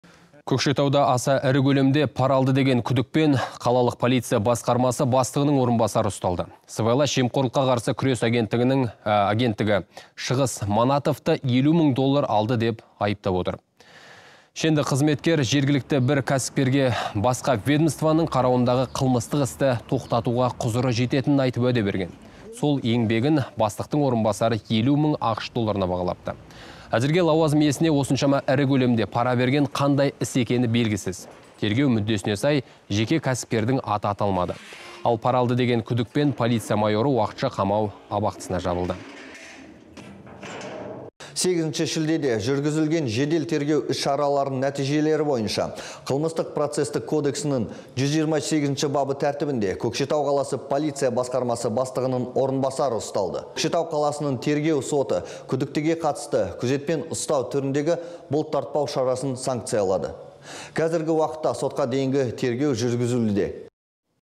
Полиция бастығы пара алған
Көкшетауда аса ірі көлемде пара алды деген күдікпен қалалық полиция басқармасы бастығының орынбасары ұсталды. Сыбайлас жемқорлыққа қарсы күрес агенттігі Шыңғыс Манатовты 50 мың доллар алды деп айыптап отыр.